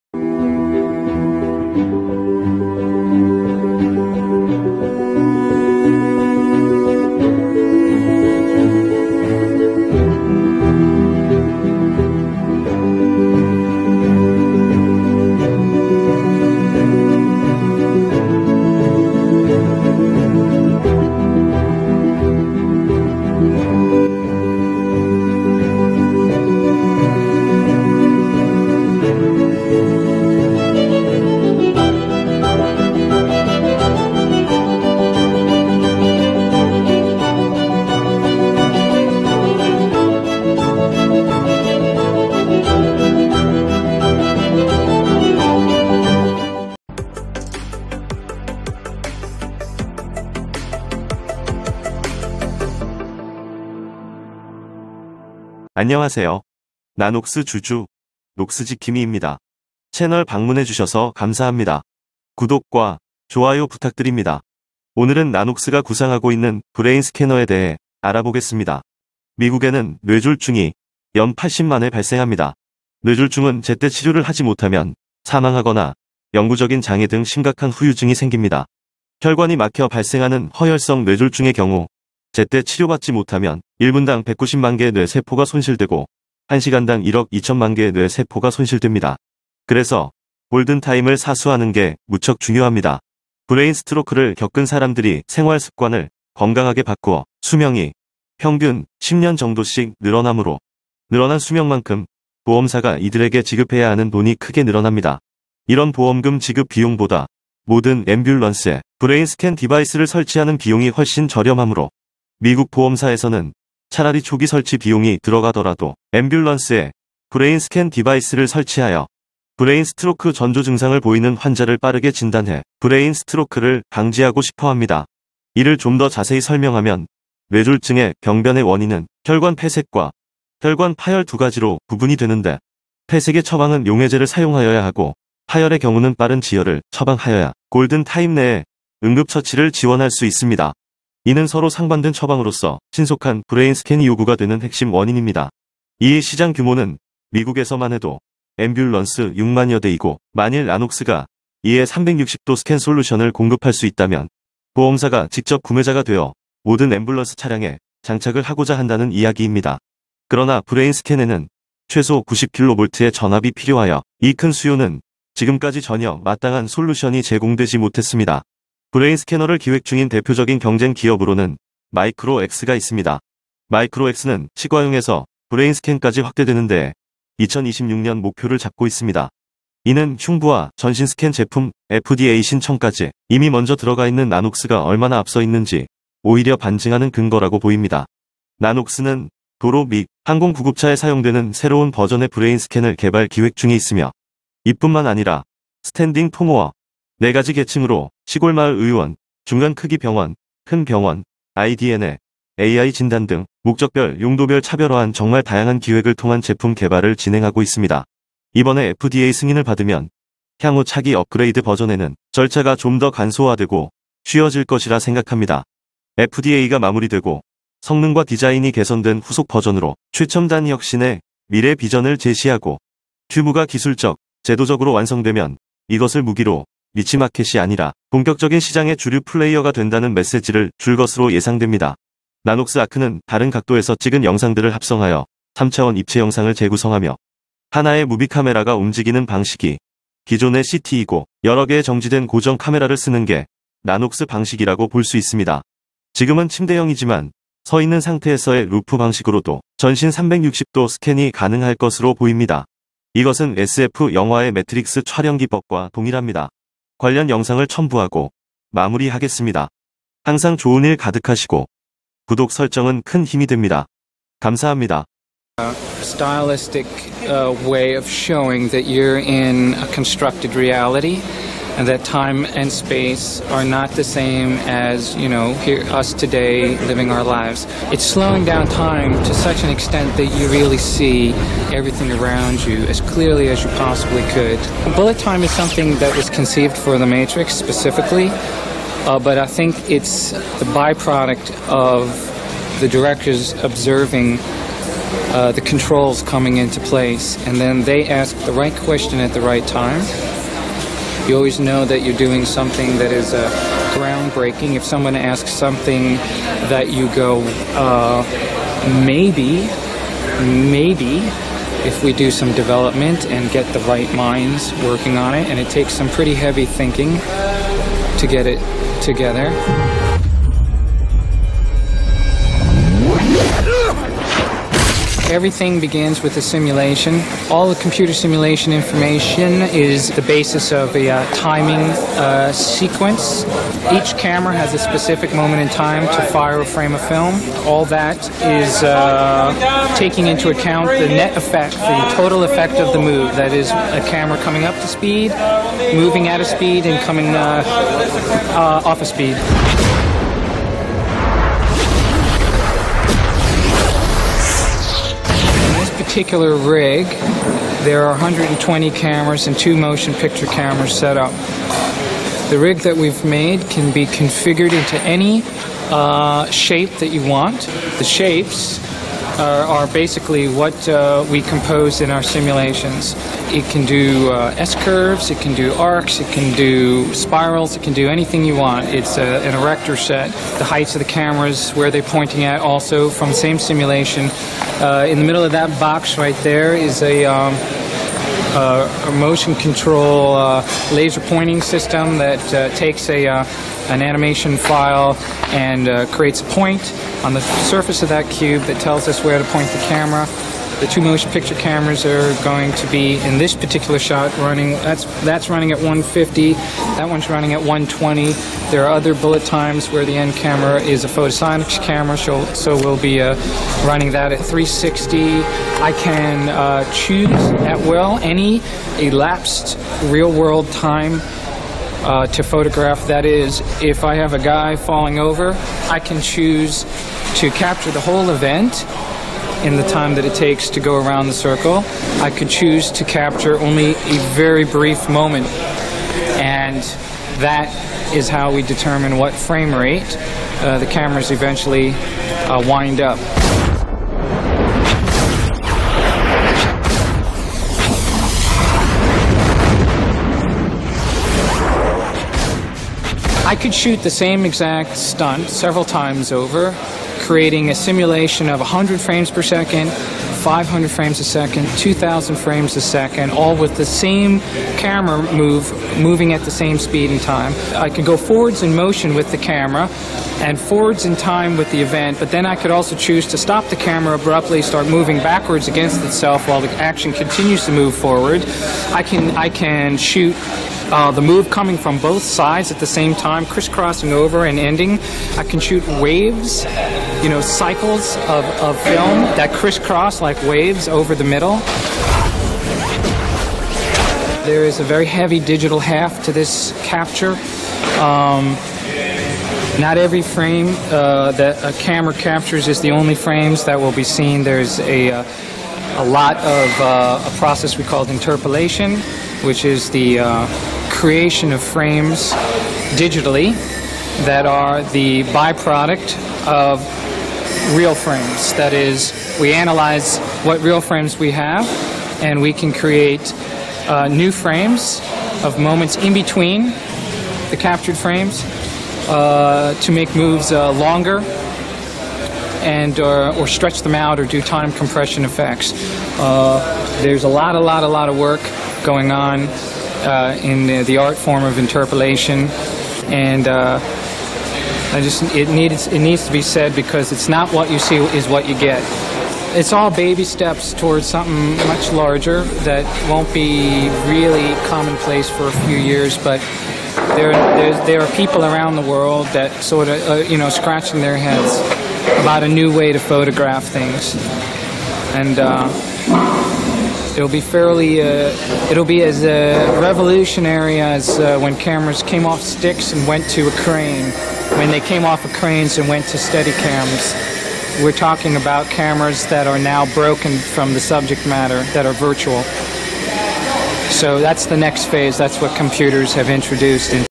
나녹스 차기 모델 Brain Scanner
나녹스는 새로운 버전의 ARC들을 준비 중에 있습니다.
예를 들면 Standing Tomo (크고 작은 제품, basic과 premium 제품 등),
4가지 계층 시골마을 의원, 중간 크기 병원, 큰 병원, IDN에, AI 진단 등 목적별 용도별 차별화한 정말 다양한 기획을 통한 제품 개발을 진행하고 있습니다.